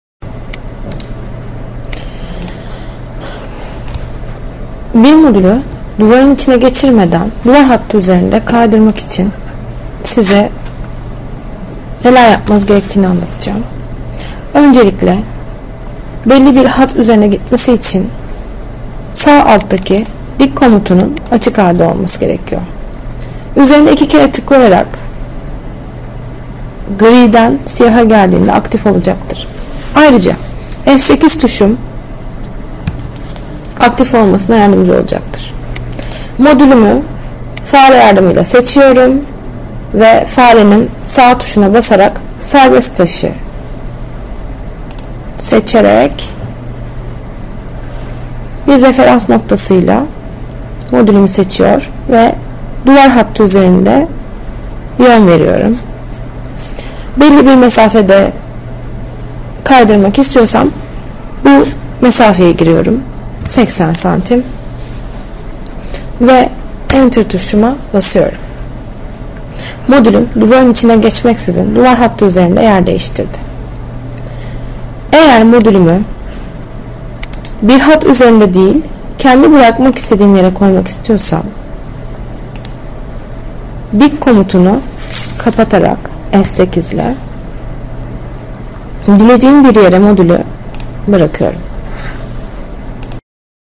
serbest tasi
Adeko'da bir objeyi yada bir modülü farklı bir yere nasıl taşırsınız?